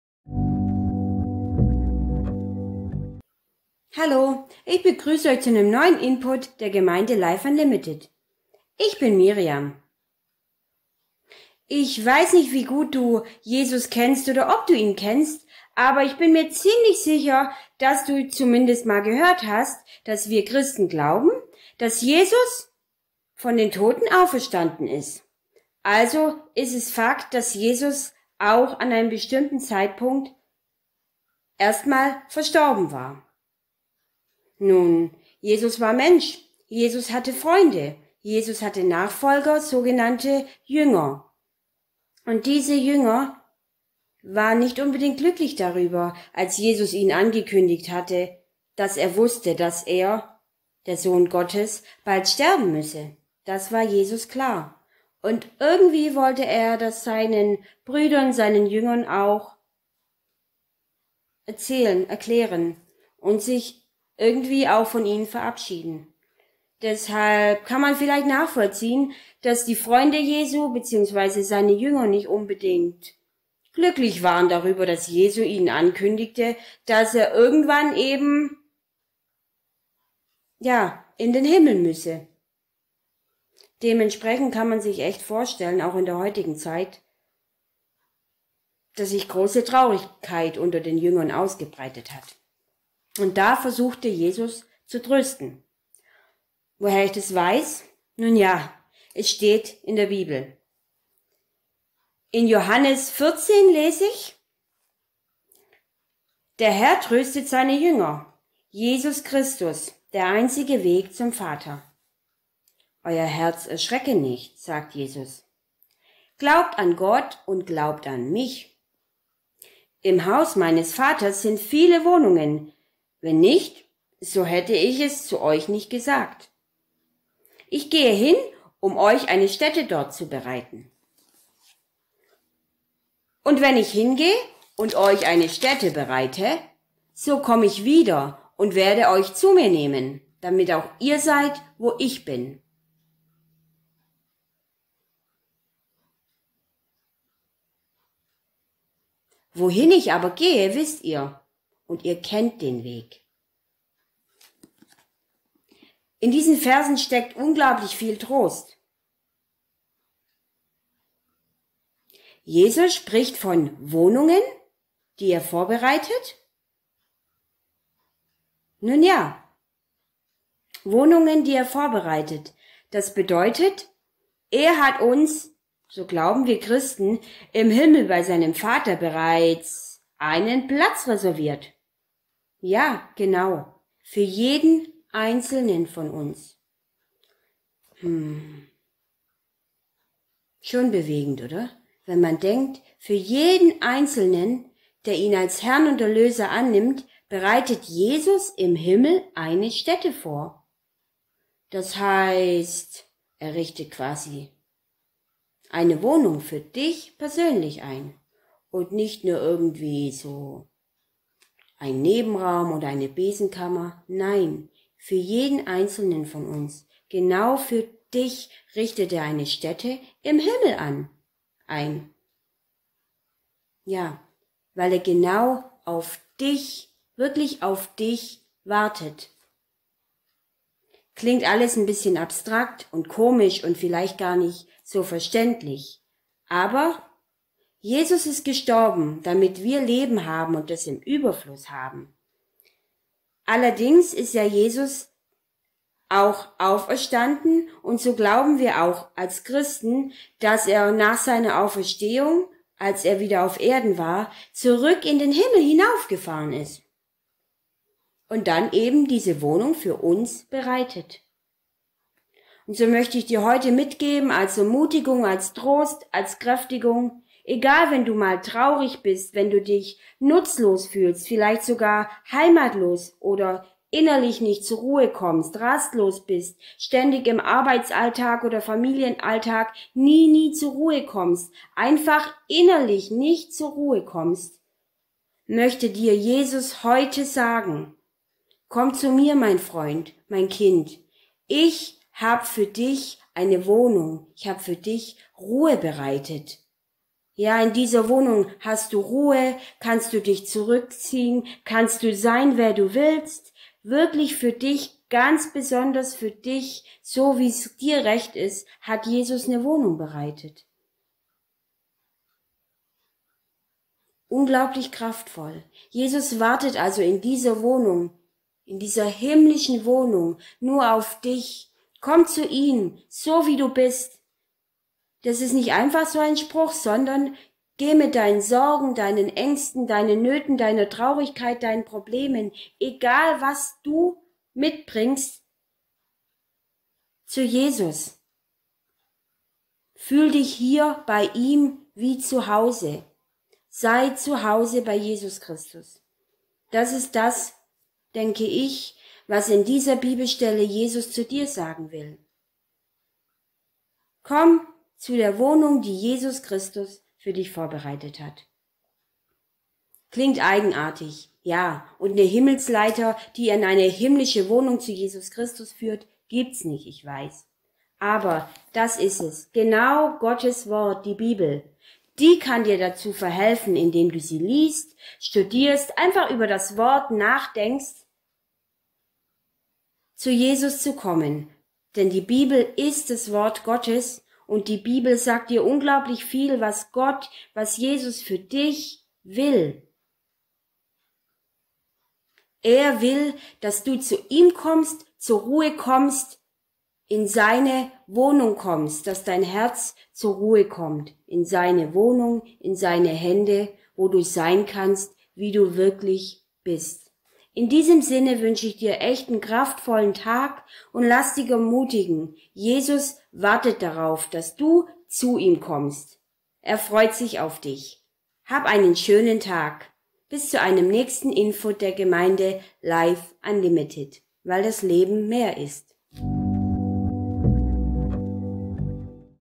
Life unlimited Input 24. Juli 2020 | Fühl dich wie zuhaus
Der Life unlimited Input am 24. Juli 2020 von Miriam. Das Thema heute: Fühl dich wie zuhaus